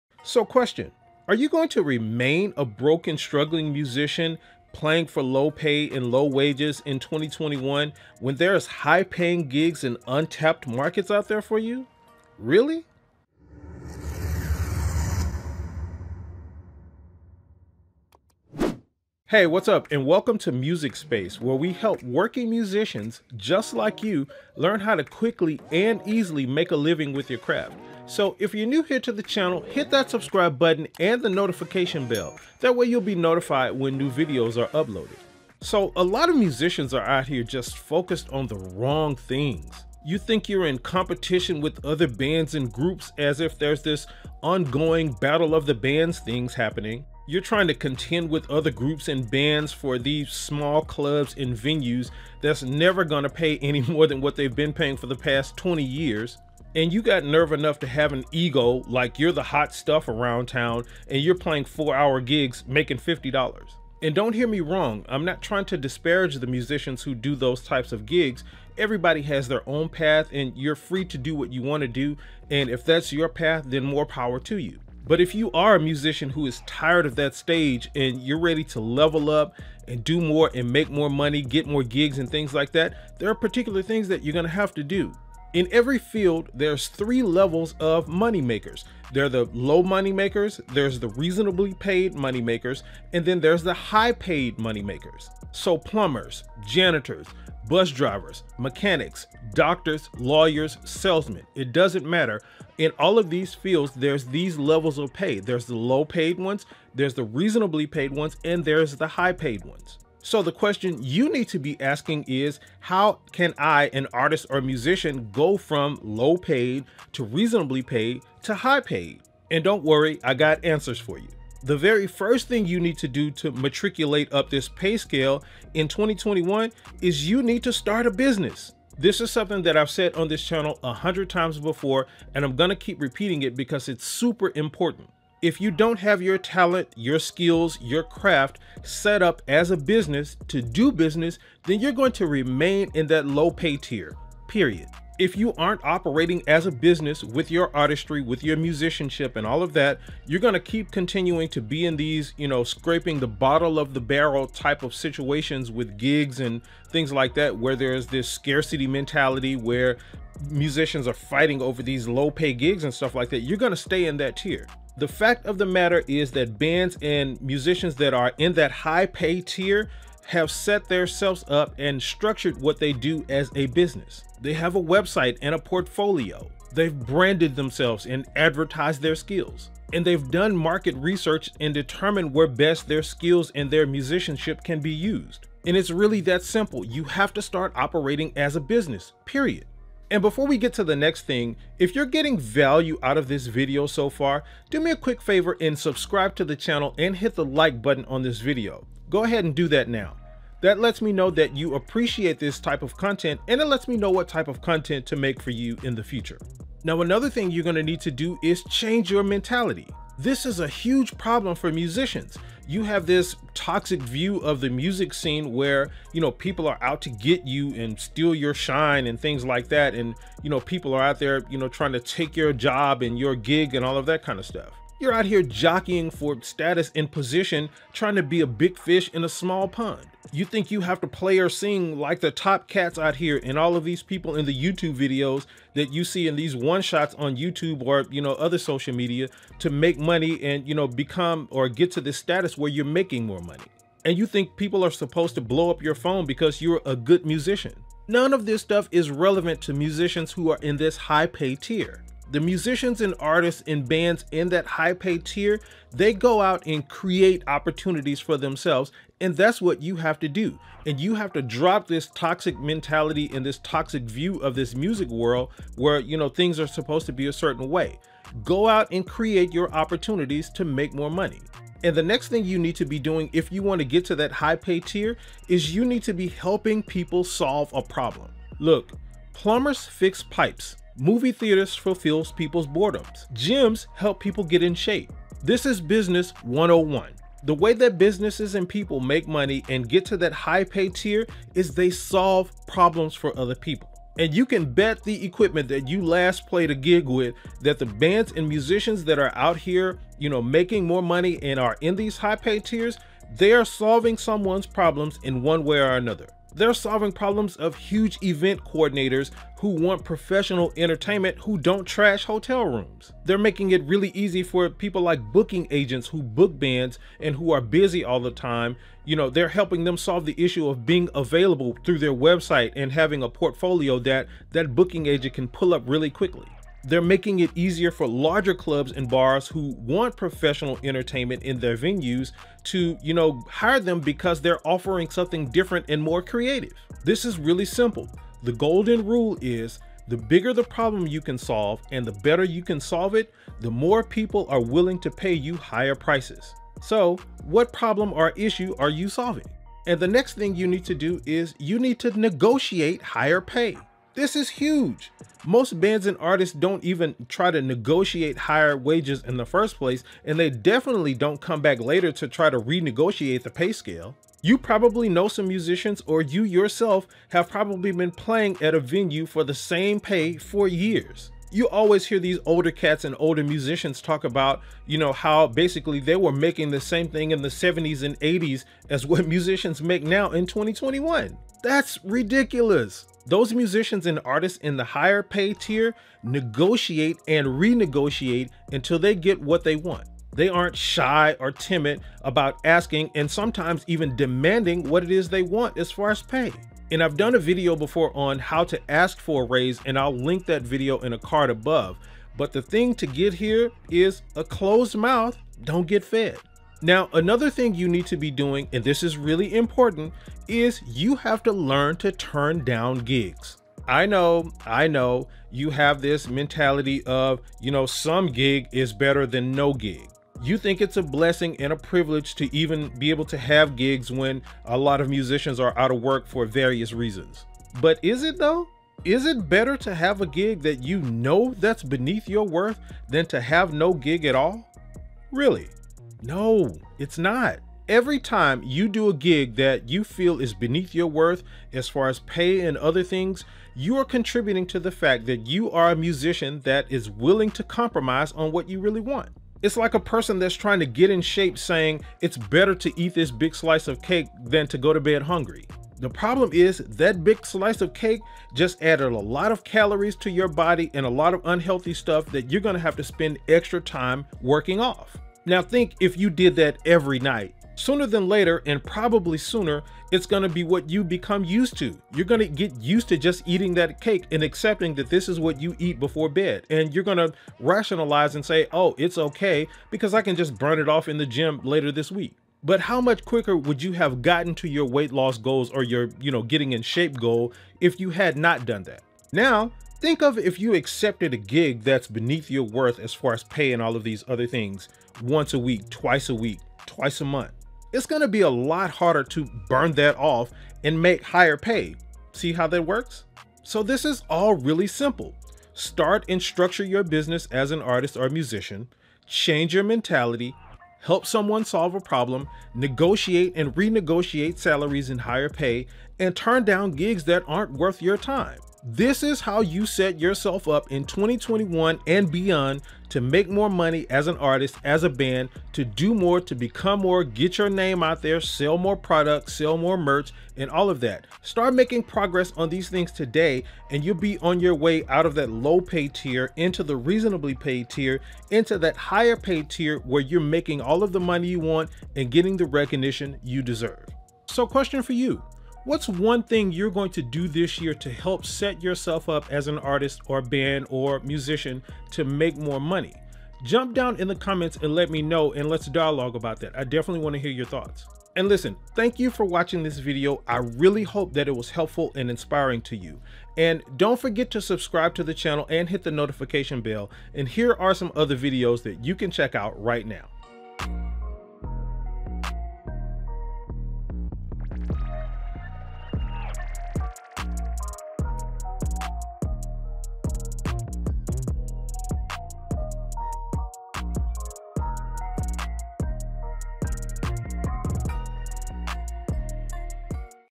How To Make More Money From Gigs In 2021
Instantly Make More Money From Gigs In 2021 By Applying These Tips

It will help bring you more valuable content. Thank you for your support!

What ONE THING are you going to do this year to set yourself up to make more money?

**Music**
Epidemic Sounds

Time Stamps
0:00 Intro
2:01 Start A Business
3:26 Change Your Mentality
4:31 Help Solve Problems
5:50 Negotiate Higher Pay
6:46 Turn Down Gigs
9:04 Question Of The Day

MakeMoreMoneyFrom Gigs